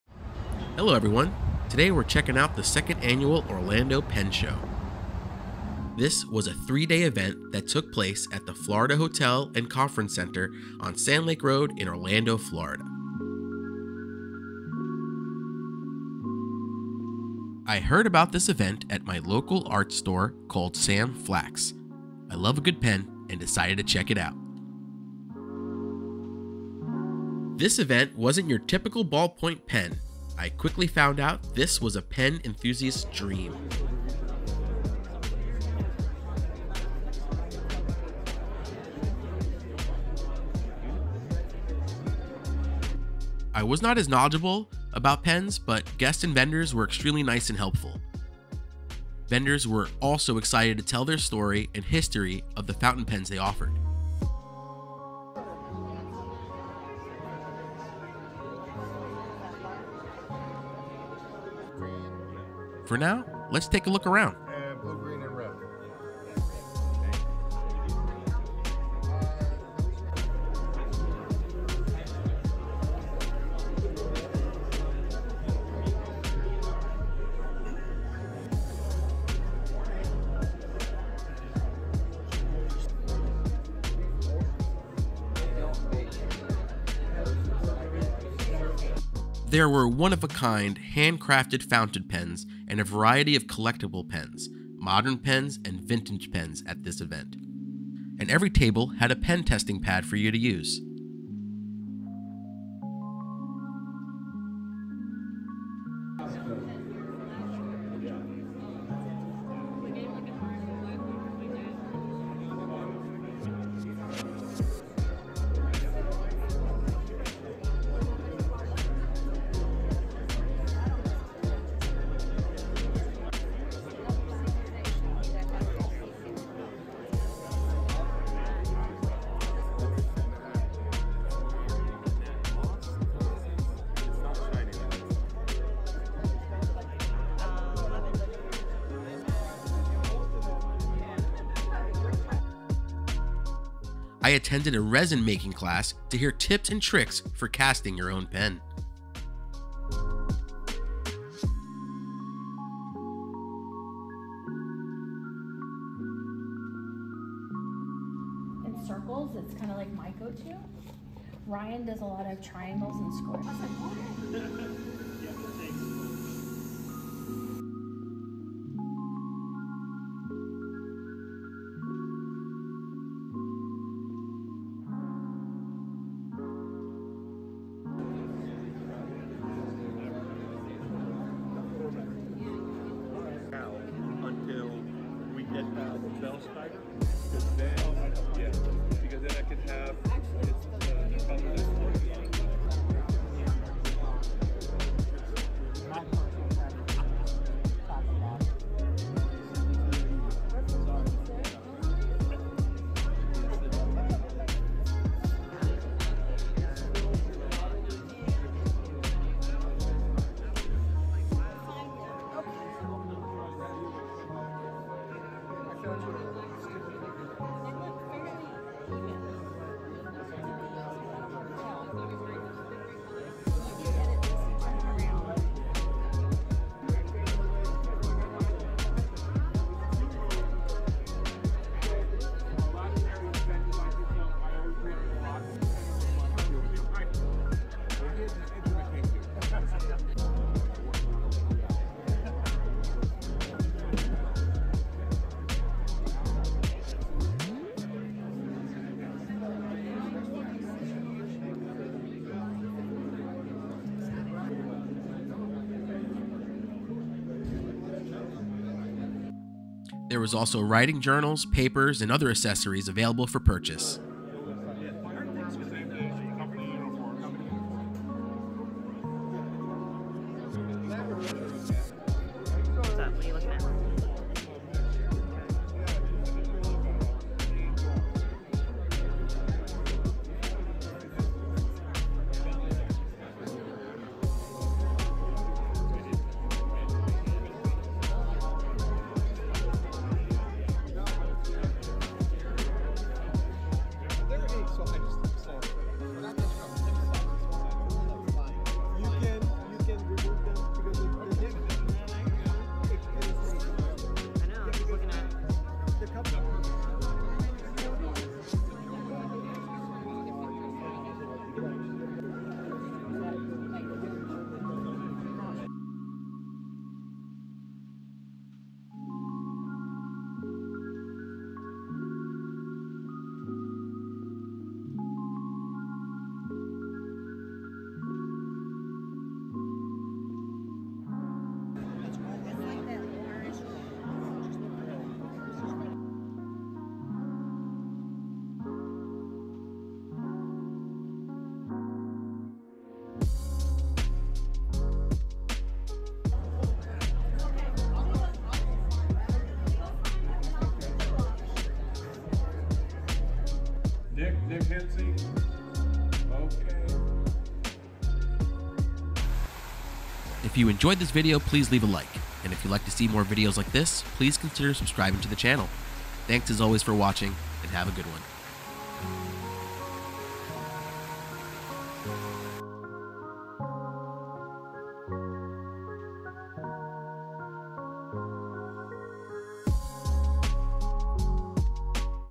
Orlando Pen Show: A Paradise for Pen Enthusiasts!
Join us as we delve into the captivating world of pens and stationery at the 2nd Annual Orlando Pen Show, taking place from September 7th to 9th 2023 at the prestigious Florida Hotel & Conference Center.

This annual event is a pen collector's dream come true, featuring a dazzling array of vintage and modern writing instruments, unique inks, paper, and accessories. Discover rare pens, meet fellow enthusiasts, and immerse yourself in the art of fine writing.

Don't miss this exciting opportunity to witness the passion and creativity of pen enthusiasts from around the world.